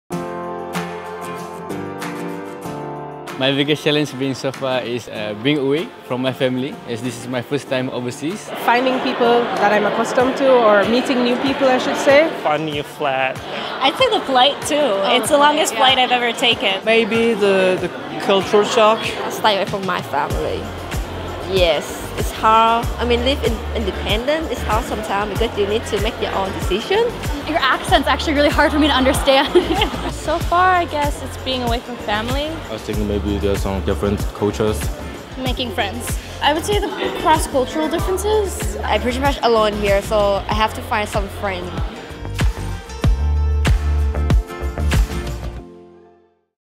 Student Experience- What's your been your biggest challenge in New Zealand so far?
Feeling nervous😱 about what to expect when you choose to StudyinNZ? How to Apply to Study in New Zealand
We went out to a New Zealand🇳🇿 institution for a day and asked real international students what their biggest challenge has been since they started their study📖 journey in New Zealand. Check out what they said⬇️.